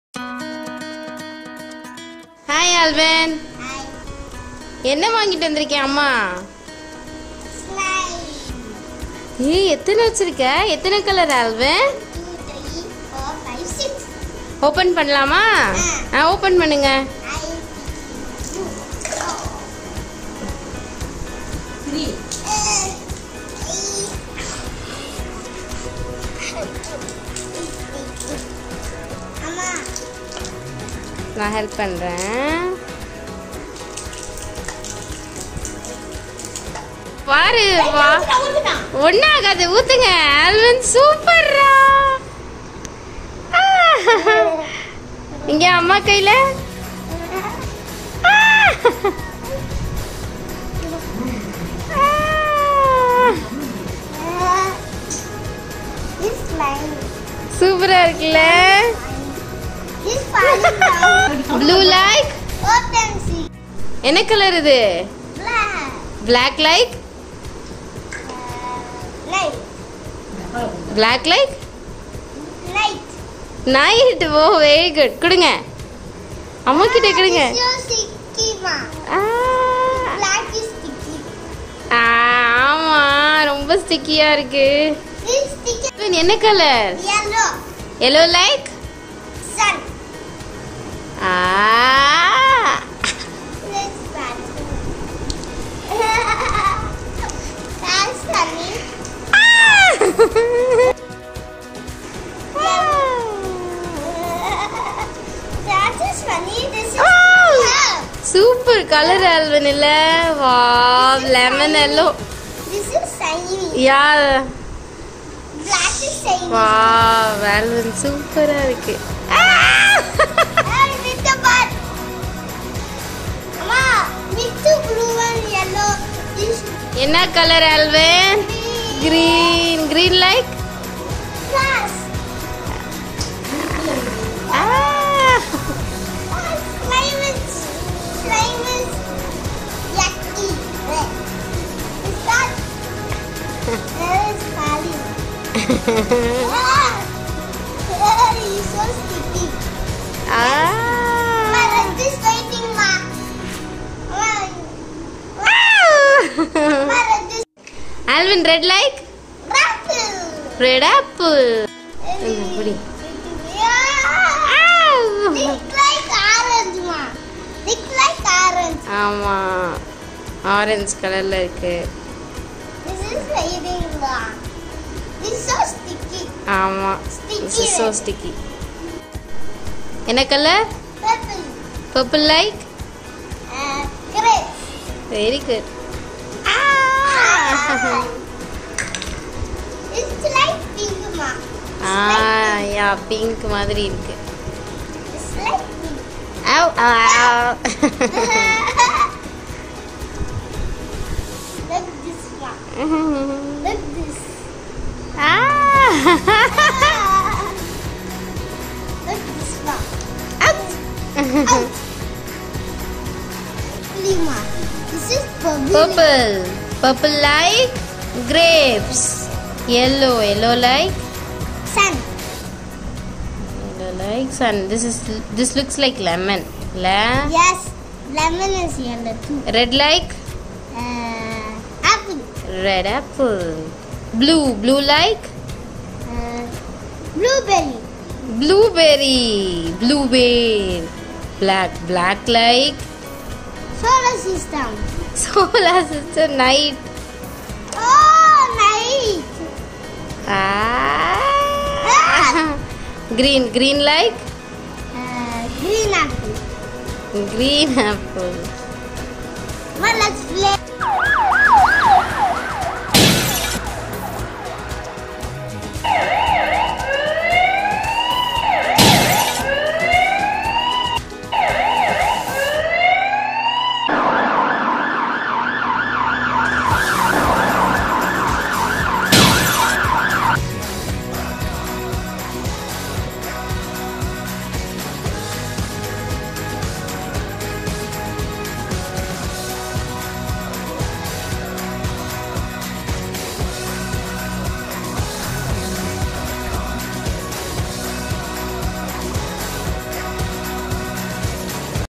Learn Colors with slime
Alvin playing with slime and learning colors in fun way